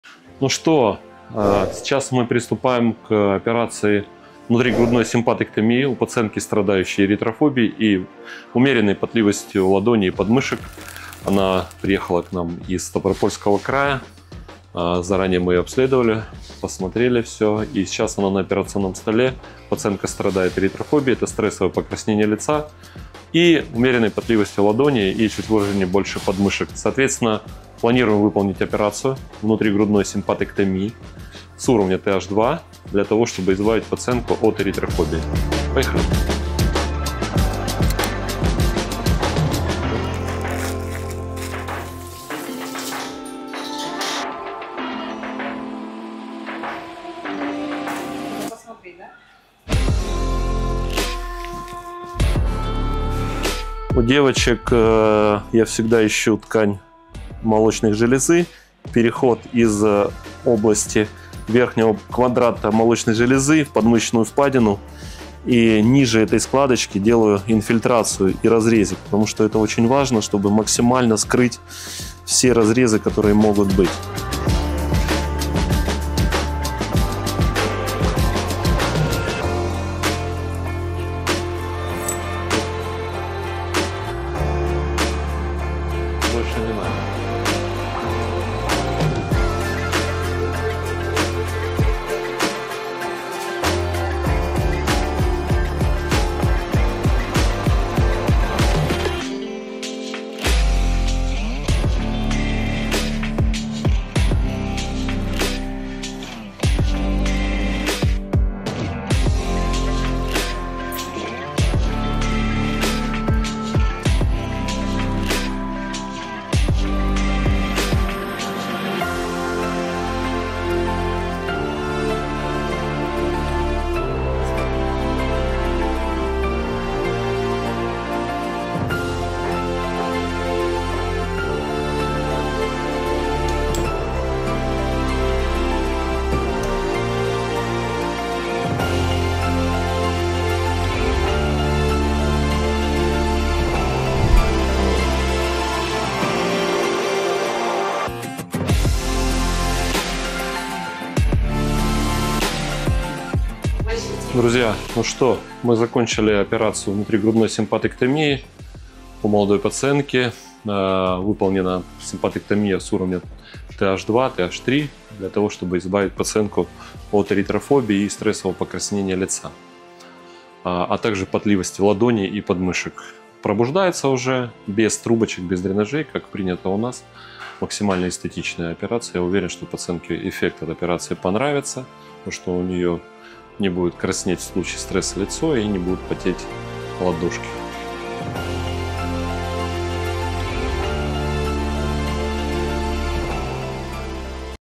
Устранение эритрофобии (блашинг синдром, стрессовое покраснение лица) у пациентки из Ставрополья
Блашинг-синдром имеет много синонимов: эритрофобия, идиопатическая эритема, стрессовая гиперемия. Заболевание проявляется внезапным патологическим покраснением лица и шеи в результате нервного напряжения, например, при необходимости публичного выступления, при общении с незнакомыми людьми и других случаях эмоционального всплеска. Очертания таких покраснений зачастую имеют неравномерные границы.

Кроме этого, эритрофобия нередко сопровождается повышенной потливостью – гипергидрозом подмышек и ладоней, так как эти изменения являются звеньями одной цепи — возбуждения симпатической нервной системы во время бодрствования.

Эта специфическая фобия связана с тревожными расстройствами. Это ненормальный и хронический страх покраснеть, который часто кажется иррациональным не только пациенту, но и окружающим. Страх связан с тем, что вы находитесь в центре внимания. Люди с социальной тревожностью также могут быть более склонны к остаточному чувству смущения или застенчивости.

Часто, если человек с эритрофобией оказывается в ситуации, когда он краснеет, он может попытаться обратить вспять свое покраснение, однако в большинстве случает это только заставляет его краснеть сильнее, создавая порочный круг.

Существует хирургическая методика, которая позволяет избавиться от стрессового покраснения — это операция внутригрудной симпатэктомии с уровня Th2.
Продолжительность операции: 40 минут
Пребывание в стационаре: 6 часов
Развитие эффекта: сразу после операции
Хирург: Маркушин Виктор Александрович